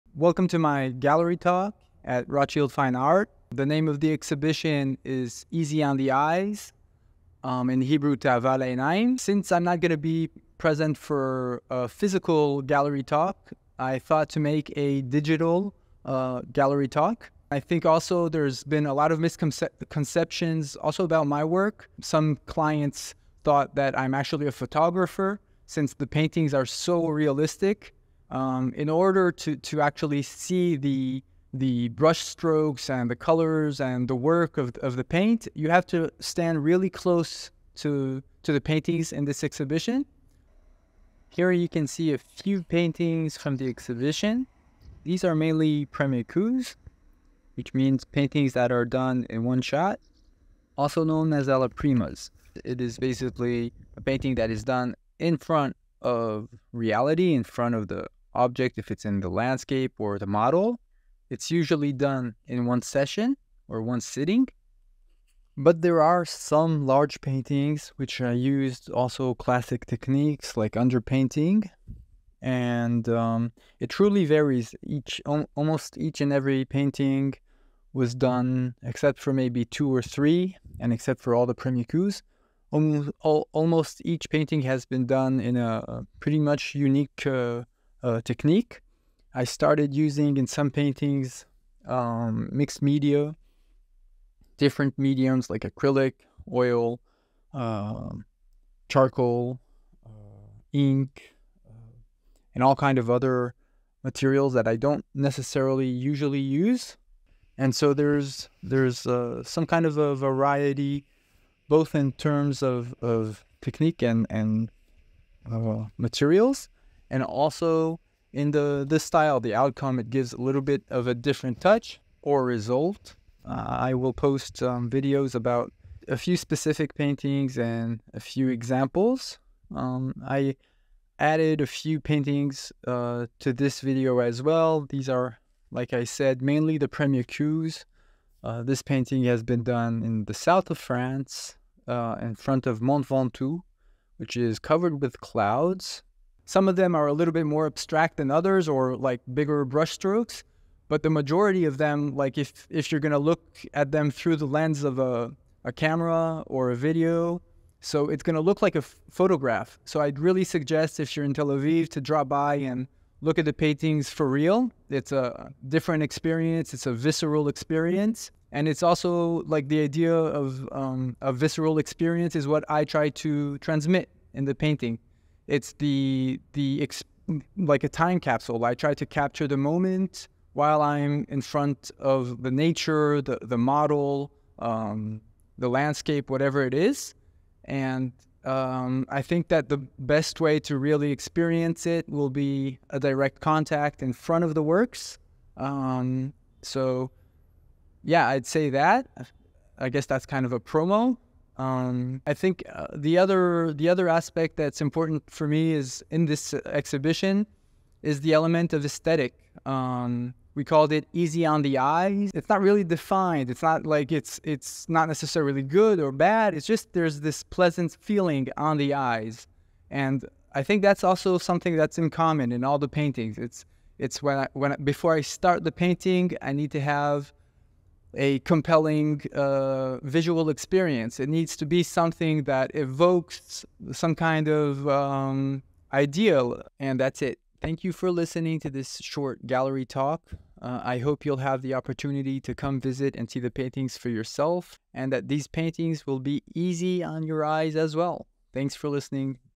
Easy on the eyes-gallery exhibition at Rothschild fine art
reality?

For many years I was imprisoned  by my imagination due to some events in my life,
I always drew painted or scribbled things... but only when I started drawing and painting from reality did I feel liberated ,I was liberated from the pre-conceived ideas, liberated from my own mind and thoughts.

Freedom was a real physical/viseral experience and at last I was in it, moreover I had a language to communicate the beauty I have seen.

the language is called painting!